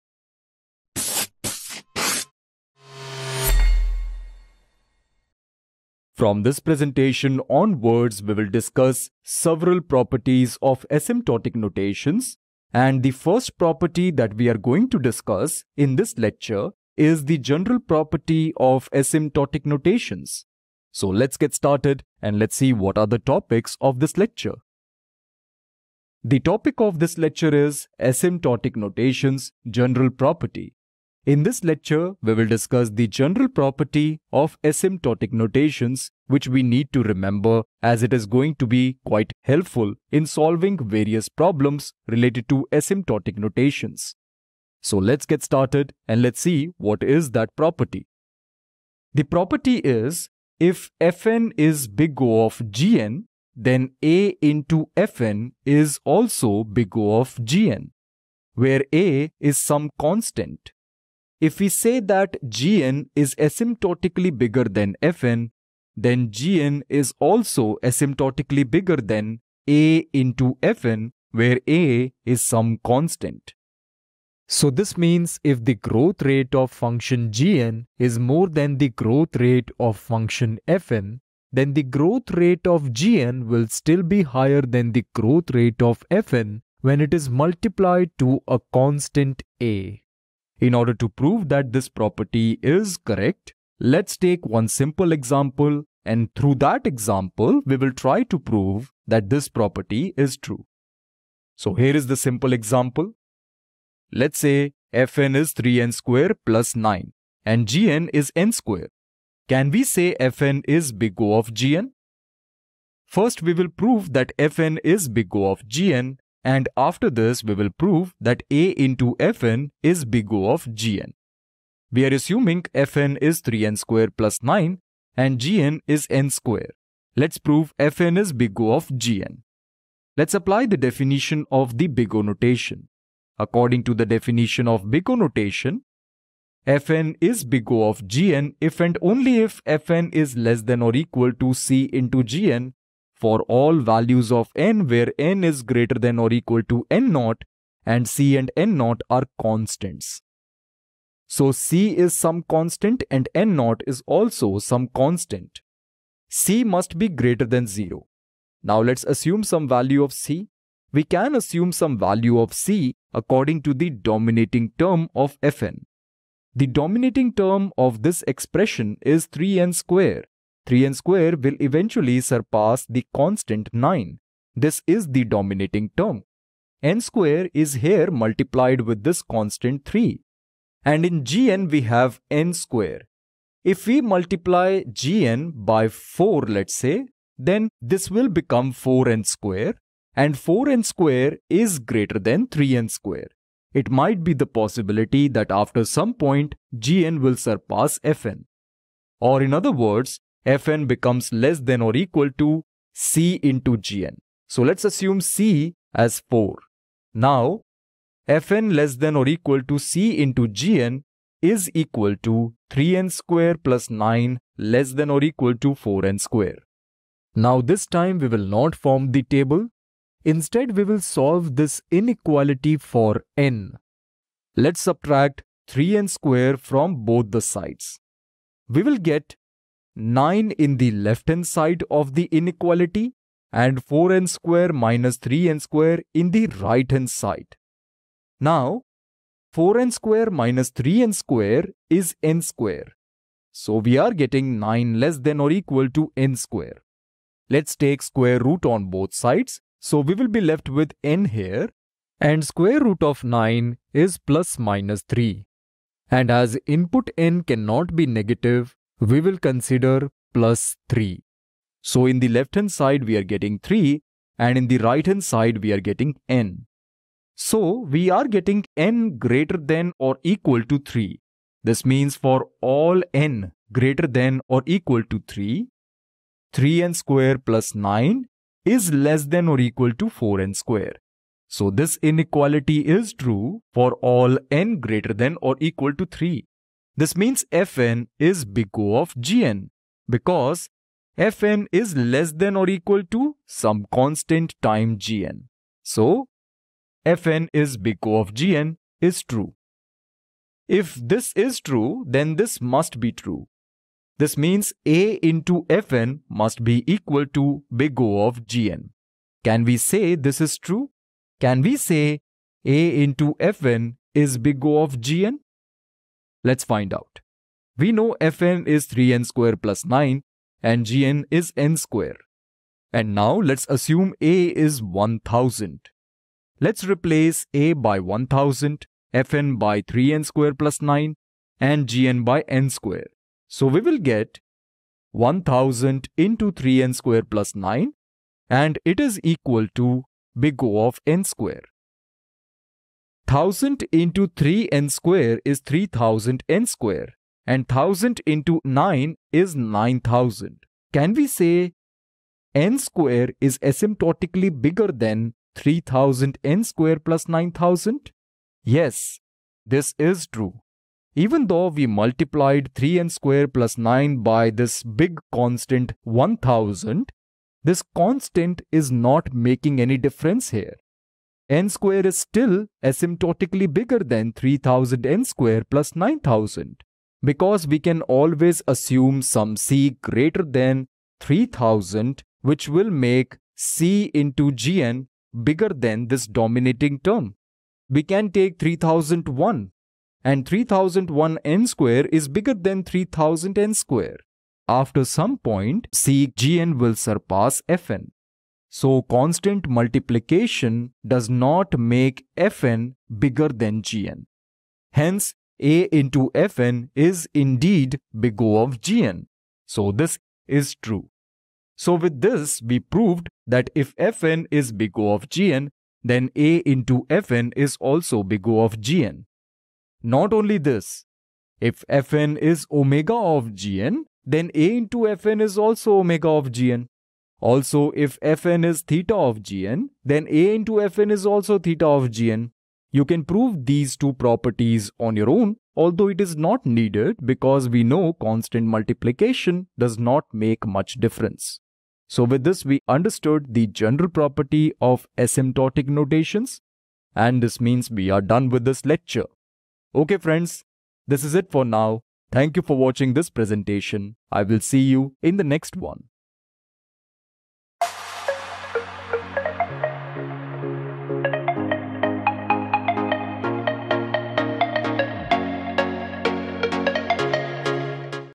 Properties of Asymptotic Notations (General Property)
Algorithms: Properties of Asymptotic Notations (General Property)
Topics discussed:
1. The General Property Property of Asymptotic Notations.